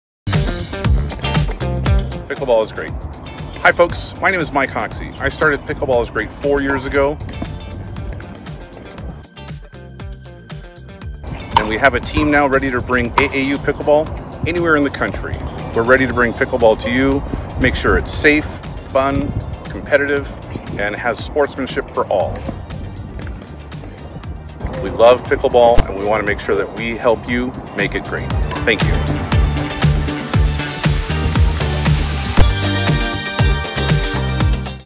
"Pickleball is Great" for AAU 4K
Mike Hoxie, owner of "Pickleball is Great" explains how his company successfully manages AAU Pickleball tournaments around the country.